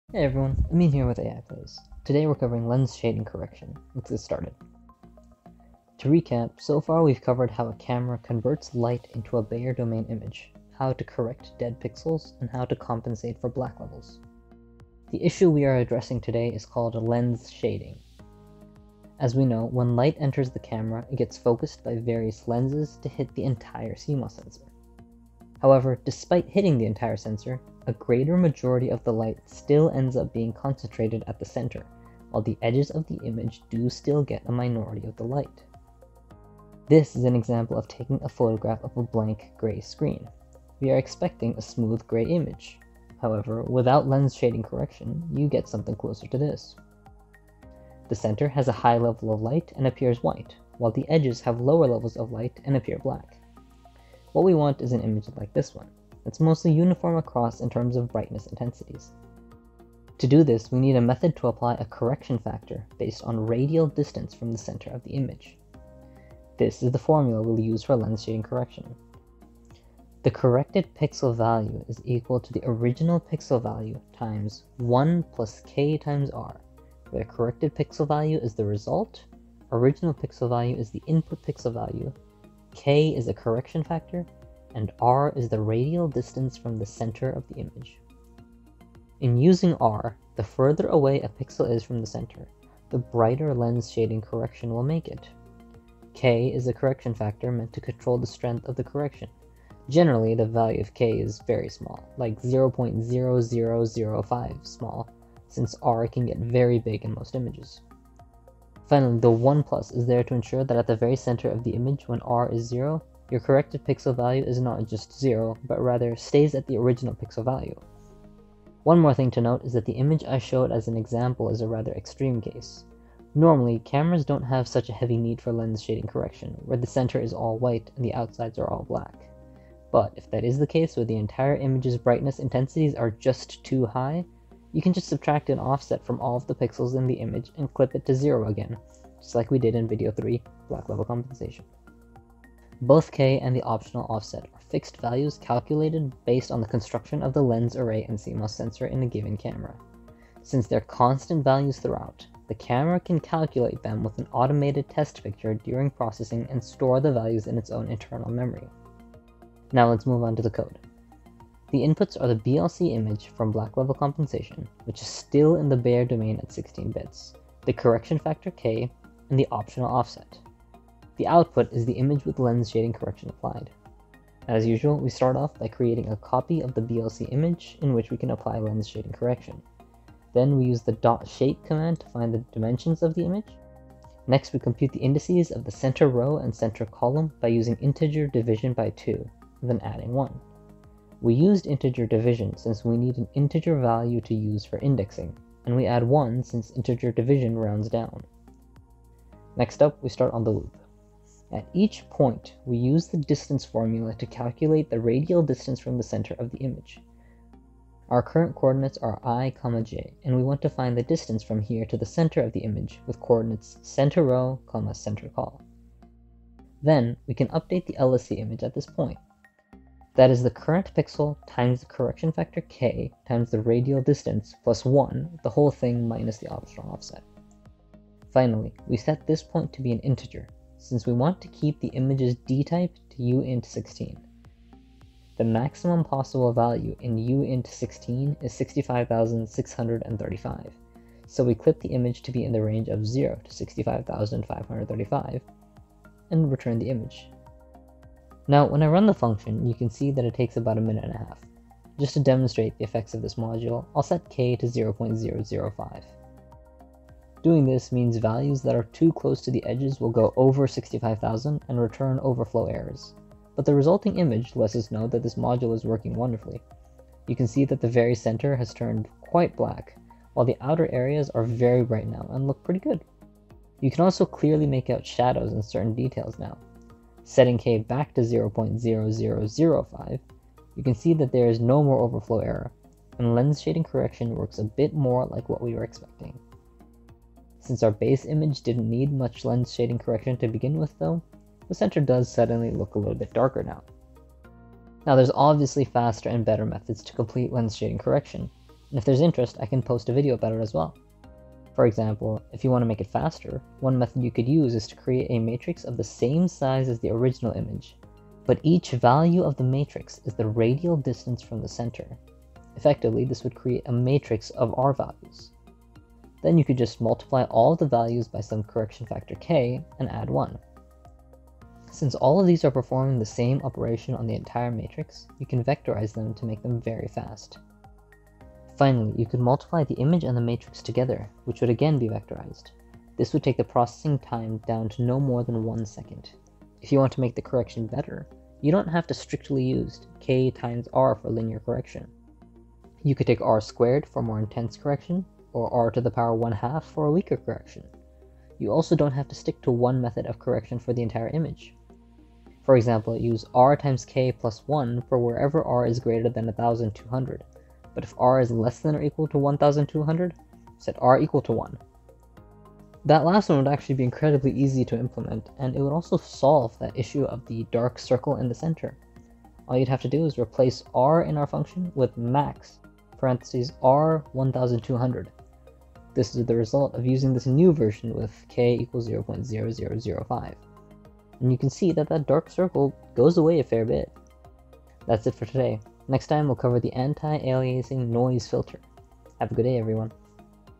(4) Lens Shading Correction | Image Signal Processing Pipeline Tutorial Series
This video covers the basics of how lens shading correction works in an image signal processing pipeline. This is the fourth video in this tutorial series.

I got all of the images from the internet to make the slides. Here is a link to a drive folder with all of the sources, as well as the Bayer domain image I will be using throughout this course:

And here is a link to my GitHub, where I post the code for each module as we go along, along with a faster version in the “Fast ISP” folder: